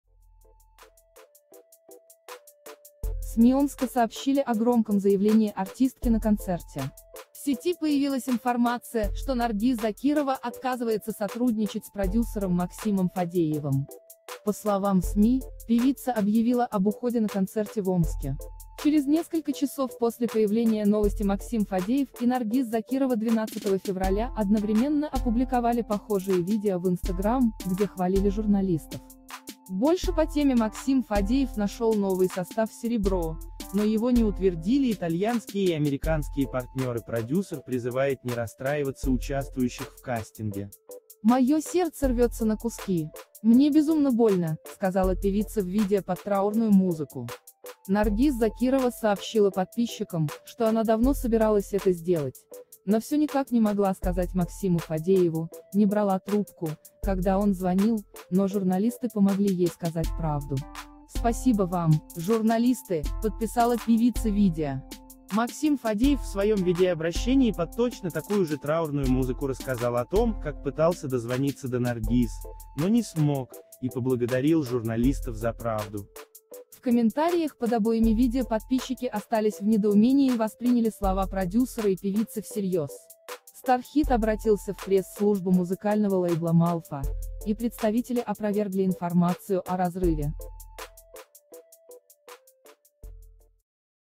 Максим Фадеев опроверг слухи об уходе Наргиз Закировой
СМИ Омска сообщили о «громком заявлении» артистки на концерте.

В Сети появилась информация, что Наргиз Закирова отказывается сотрудничать с продюсером Максимом Фадеевым. По словам СМИ, певица объявила об уходе на концерте в Омске. Через несколько часов после появления новости Максим Фадеев и Наргиз Закирова 12 февраля одновременно опубликовали похожие видео в Instagram, где хвалили журналистов.

больше по теме Максим Фадеев нашел новый состав Serebro, но его не утвердили итальянские и американские партнеры Продюсер призывает не расстраиваться участвующих в кастинге.

«Моё сердце рвётся на куски. Мне безумно больно», — сказала певица в видео под траурную музыку. «Спасибо вам, журналисты», — подписала певица видео.

Максим Фадеев в своём видеообращении под точно такую же траурную музыку рассказал о том, как пытался дозвониться до Наргиз, но не смог, и поблагодарил журналистов «за правду». Starhit обратился в пресс-службу музыкального лейбла MALFA, и представители опровергли информацию о разрыве.